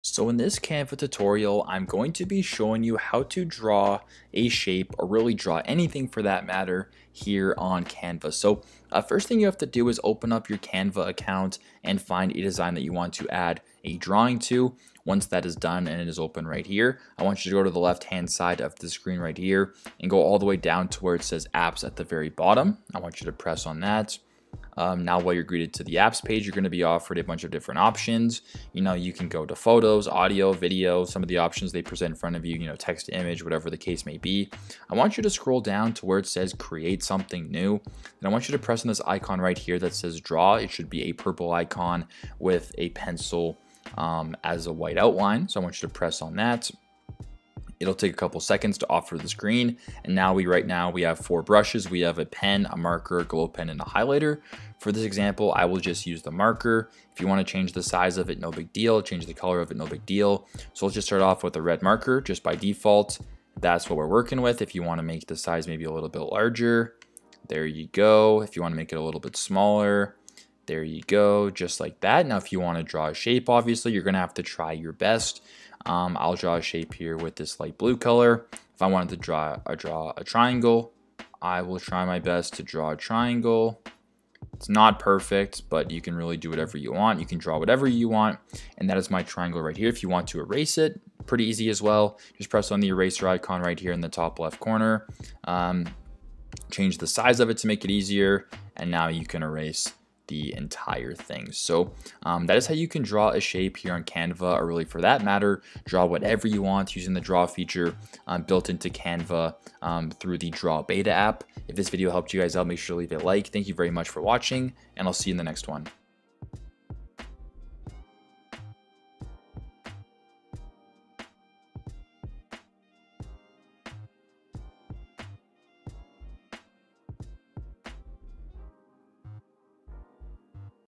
How To Draw A Shape In Canva | Canva Tutorial
In this Canva tutorial, I will be showing you how to draw a shape in Canva. Drawing a shape in Canva is a straightforward process. Here's how to do it:

1. Open your Canva design and click on the "Shapes" tab on the left-hand side of the editor.

2. Select the shape you want to draw from the list of options. Canva offers a wide range of shapes, including rectangles, circles, triangles, lines, and more.

3. Click and drag your mouse on the canvas to draw the shape. Canva will automatically create the shape based on the size and shape of your mouse cursor.

4. Once you have drawn the shape, you can customize it by changing its size, color, stroke width, and other properties.

5. To change the size of the shape, click on one of the handles and drag it to resize the shape.

6. To change the color of the shape, click on the color picker tool and select a new color from the palette.

7. To change the stroke width of the shape, click on the stroke width tool and adjust the slider.

8. You can also add other elements to the shape, such as text or images, by dragging them onto the shape.

9. That's it! You have successfully drawn a shape in Canva. Note that Canva also offers pre-designed shapes and shapes with cutouts, which you can customize further to fit your design needs.